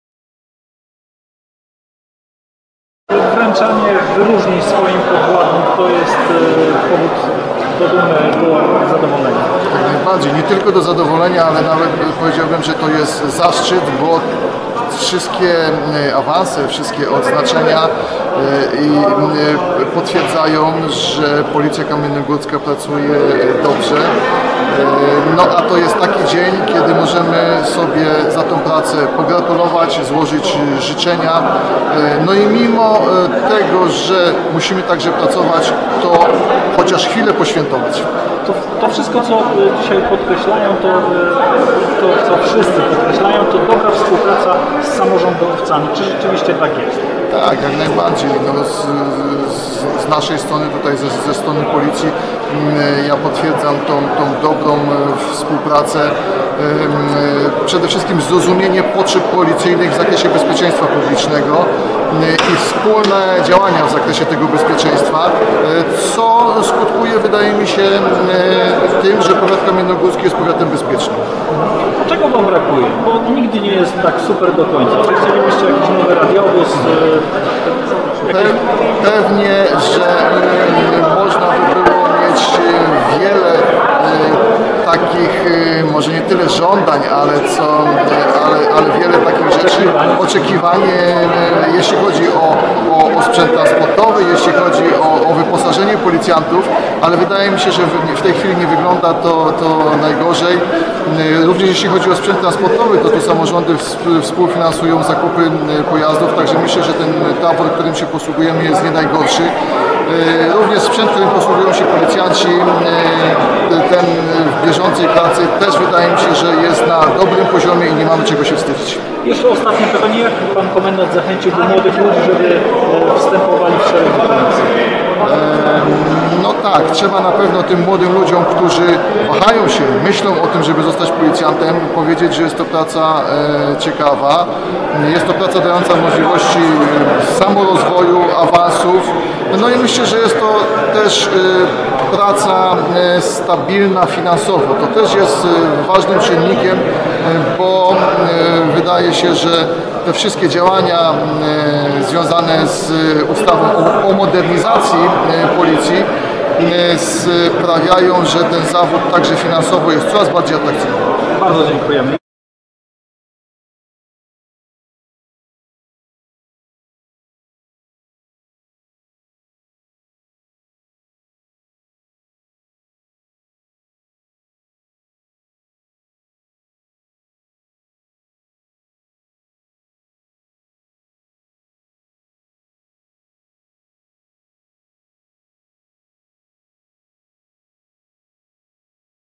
Święto Policji w Kamiennej Górze
Rozmowa z Komendantem Powiatowym Policji w Kamiennej Górze insp. Januszem Cygalem, po uroczystościach z okazji 100. rocznicy utworzenia Policji Państwowej. Reportaż fotograficzny z wydarzenia dostępny tutaj